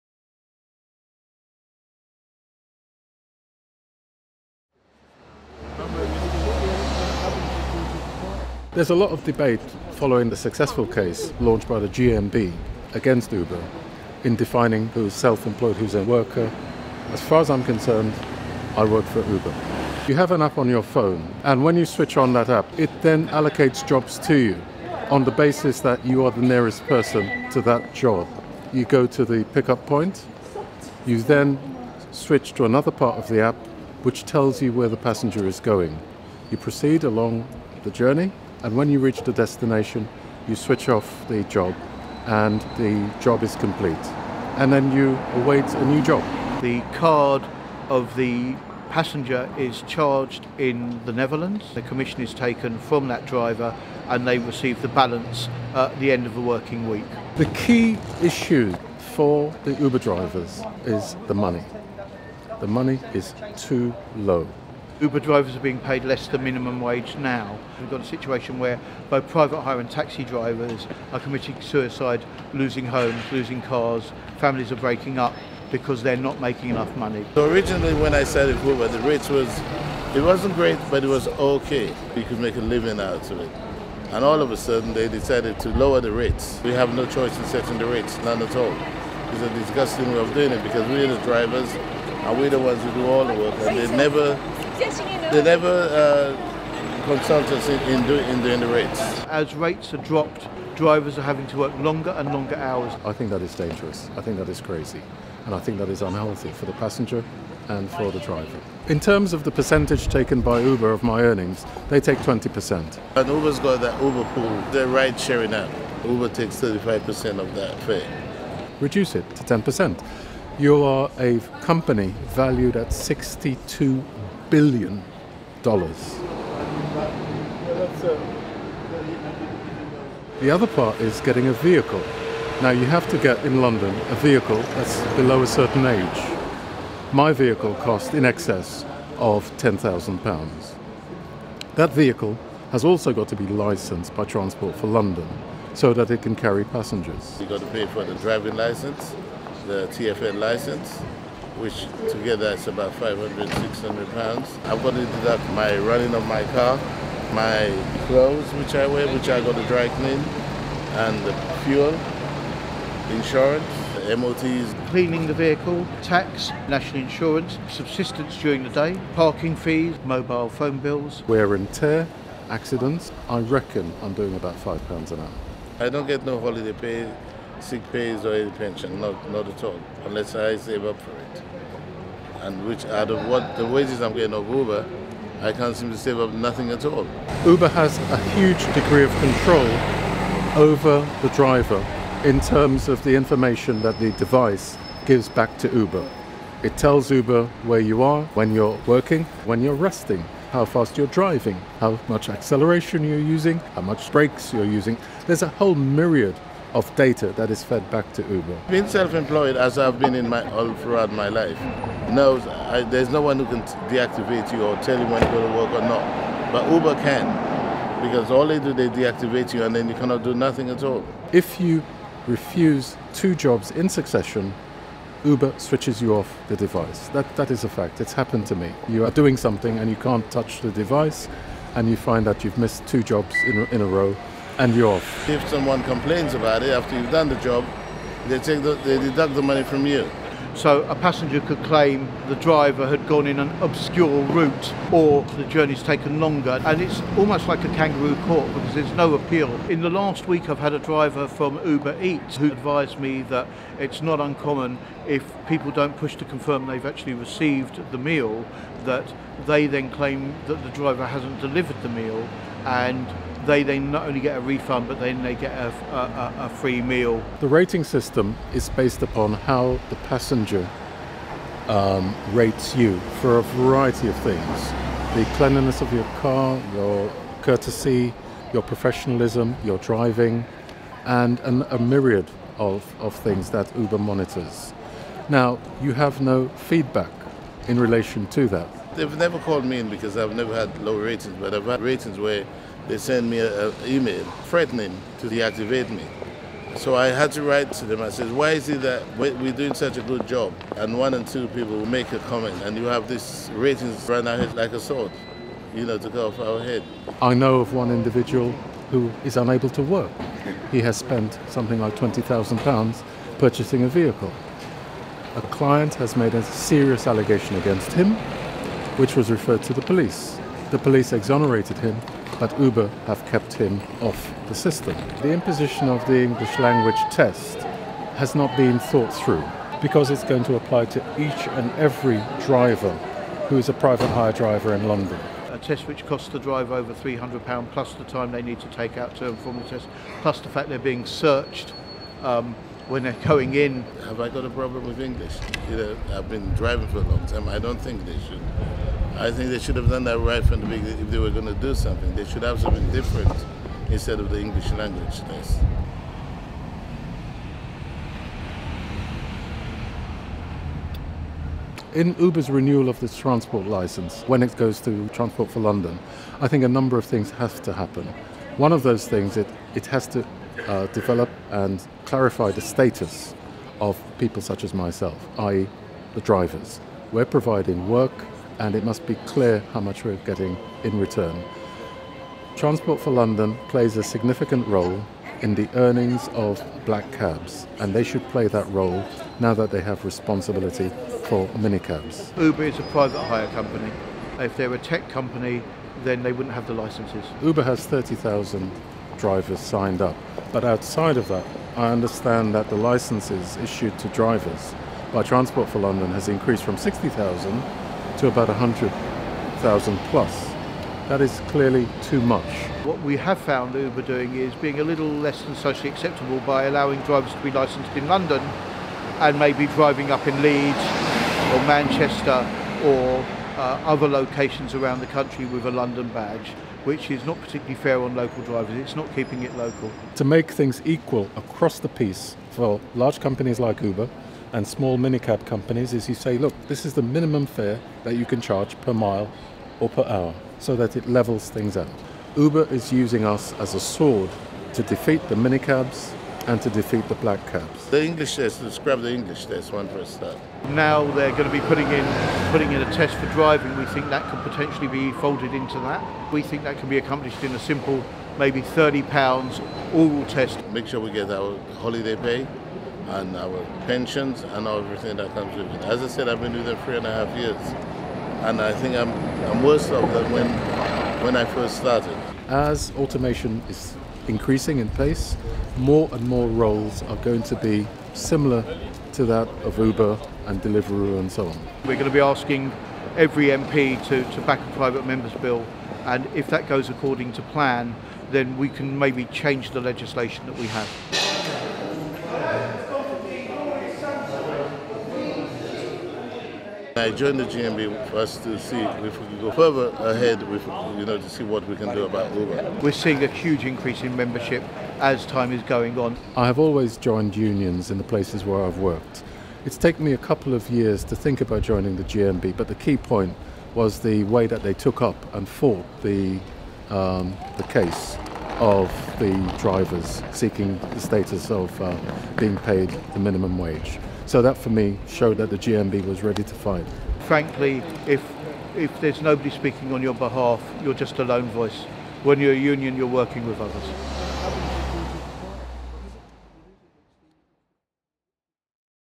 Are Uber drivers treated fairly?
As Uber's licence with Transport for London comes up for renewal, three drivers talk about all the things TfL need to take into account - Uber are not paying a living wage, do not cover expenses, don't pay sick pay, holiday or pensions, and punish workers for not taking jobs. All of this needs to be sorted out before Uber can continue operating.